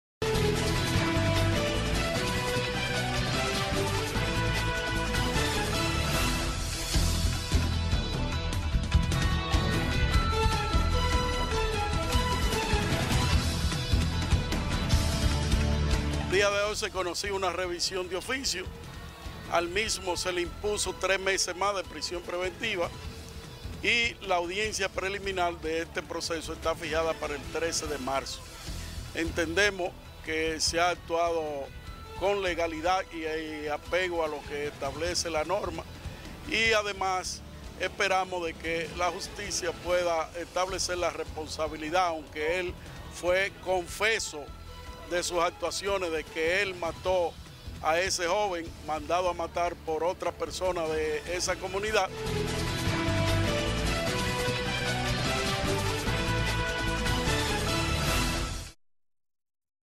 Renuevan por tres meses prisión de preventiva contra acusado de ultimar joven en sector San Martín d
AL FILO DE LAS NOTICIAS.- SAN FRANCISCO DE MACORÍS. Por tres meses más le fue renovada la prisión preventiva dictada al joven Erick Manuel Núñez Paulino, acusado de ultimar a tiros al joven Ángel Gabriel Marizán, conocido como Lubriel, de unos 25 años de edad, en un hecho ocurrido el 26 de agosto del pasado año 2016 en el sector San Martín de Porres de esta ciudad.

La Juez del Primer Juzgado de la Instrucción del Distrito Judicial de la Provincia Duarte, Yudelka Buret, envió por tres meses mas al Centro de Corrección y Rehabilitación Vista Al Valle, para continuar con la investigación del hecho.

Núñez Paulino habría fingido poseer menoría de edad, según explica el abogado defensor de la parte querellante, licenciado Freddy Fañas.

Fañas indicó que el 13 de Marzo del presente año se conocerá la audiencia preliminar para discutir los medios de prueba.

Se recuerda que en la pasada audiencia, cuando le fue impuesta la prisión preventiva, el imputado se declaró culpable de los hechos. “Me declaro culpable”, dijo.

Por el momento las autoridades no han ofrecido una versión concreta de los hechos.